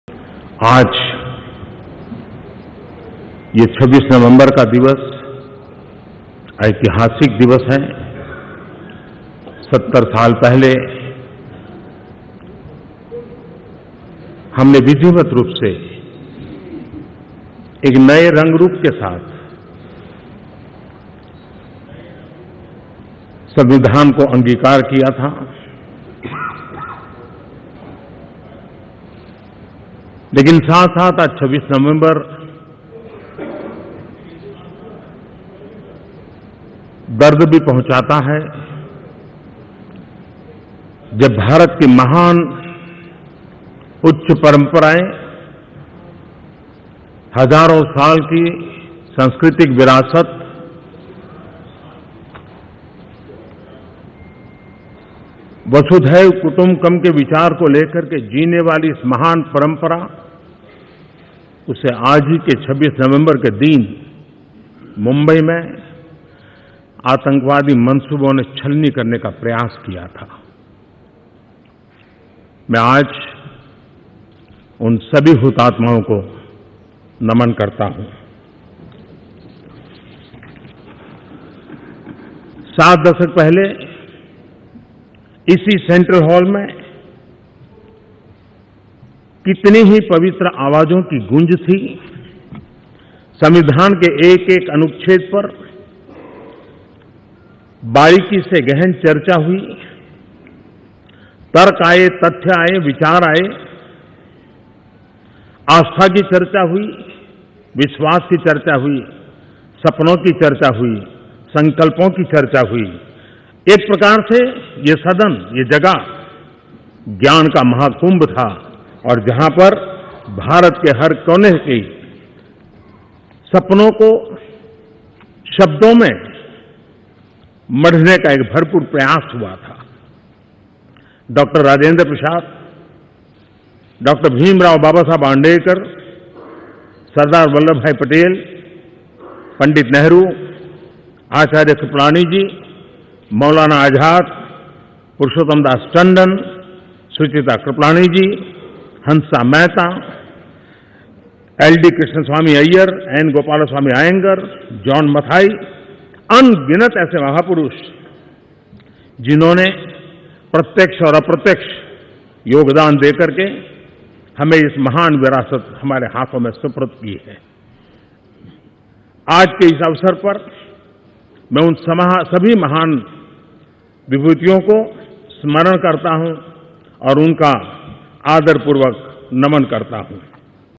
संविधान दिवस के मौके पर PM MODI ने संविधानकर्ताओं को किया नमन
संविधान दिवस के मौके पर पीएम मोदी ने लोकसभा से संविधानकर्ताओं को नमन किया... और साथ ही उन सभी नेताओं के बारे में बतलाया जिन्होंने संविदान बनाने में अहम योगदान निभाया था...